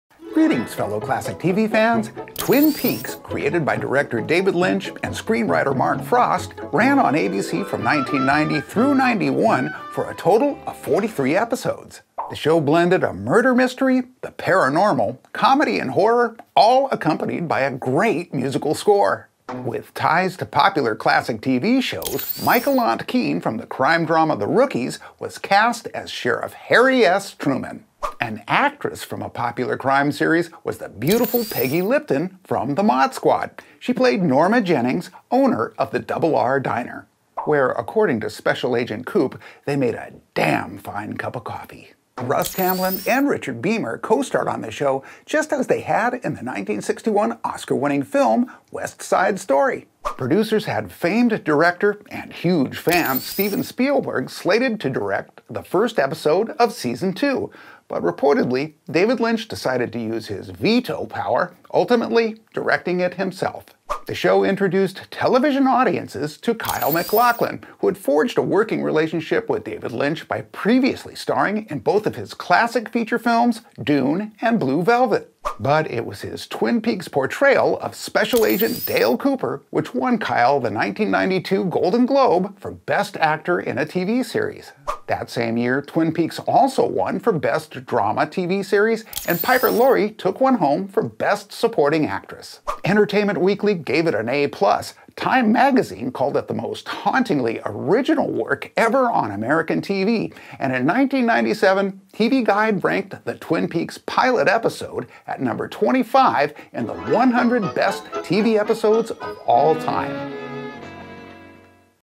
Twin Peaks! A Mystery, Horror, Sci-Fi, Comedy, Drama Cocktail!
The directing style of David Lynch made this show captivating, cool, and absolutely terrifying all at the same time. No one can deny the sheer talent of the cast as well. And in true Hitchcock form, Lynch even cast himself! It definitely made me want to try cherry-pie and fly-fishing. Not together of course.
It's interviews with some of our favorite classic tv celebrities!
You'll also find it available on most of your favorite podcast platforms.